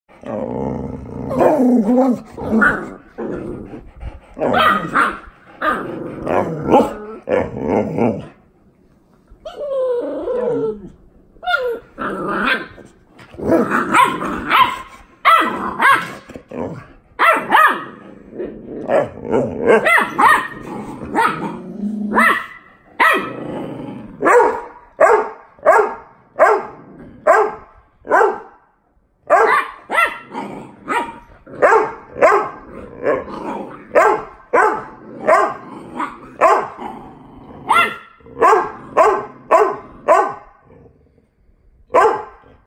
BOXER PUPPY VS BIG SISTER!
Cute boxer puppy fighting with his big sister!